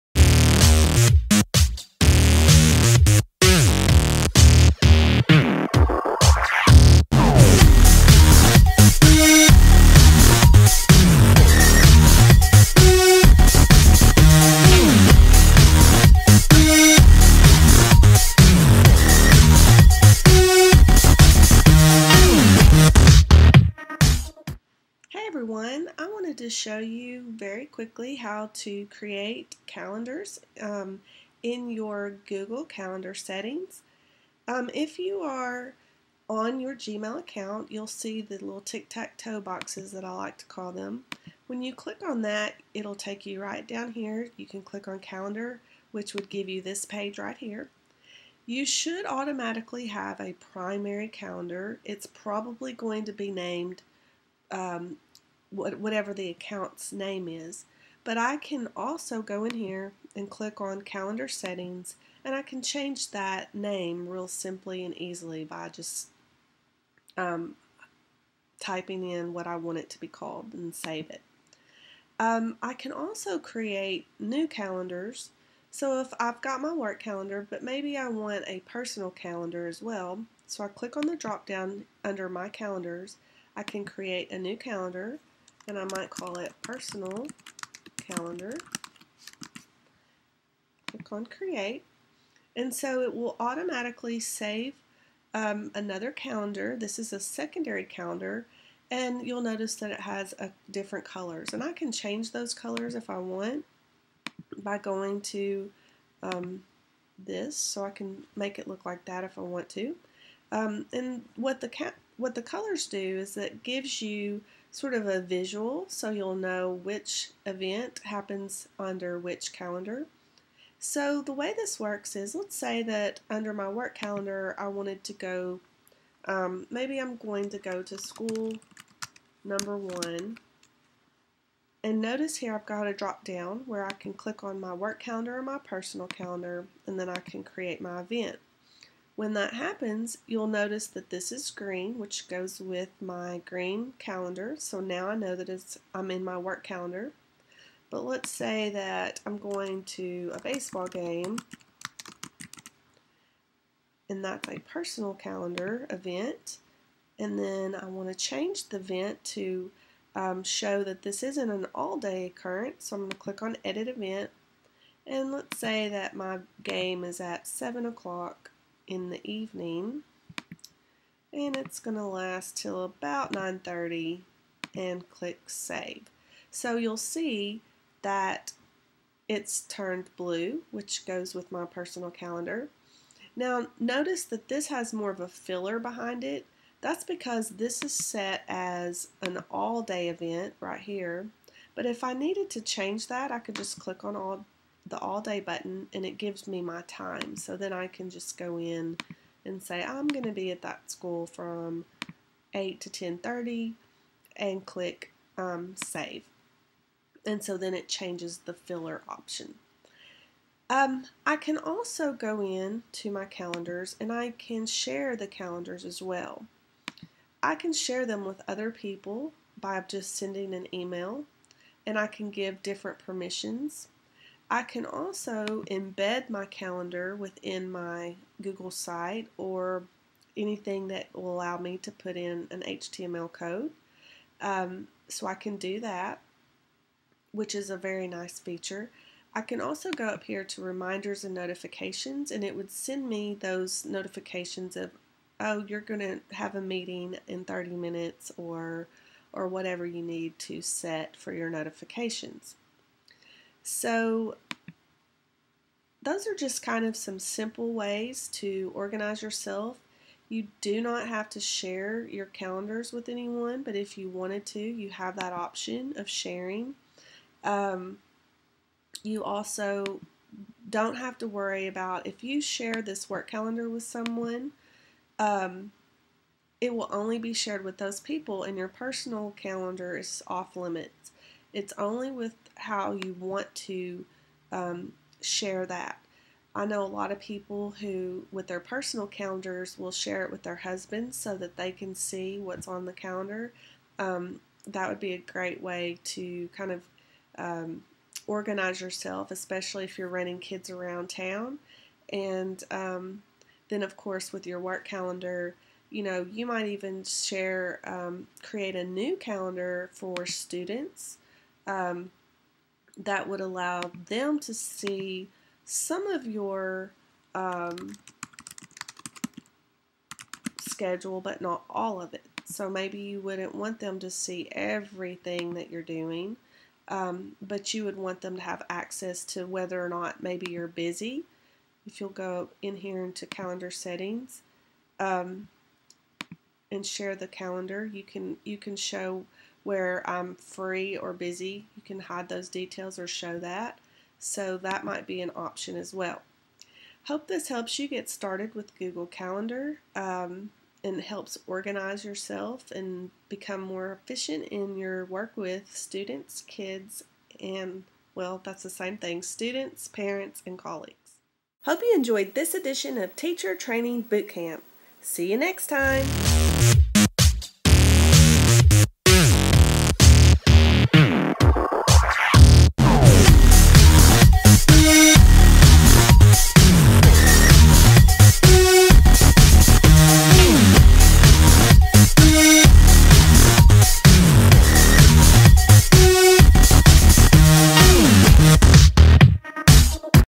Teacher Training Bootcamp: Google Calendar
Learn how to create, view, and share calendars to make your work with parents, colleagues, and students more efficient. Join us for other great calendar ideas at our Teacher Training Bootcamp Linky Party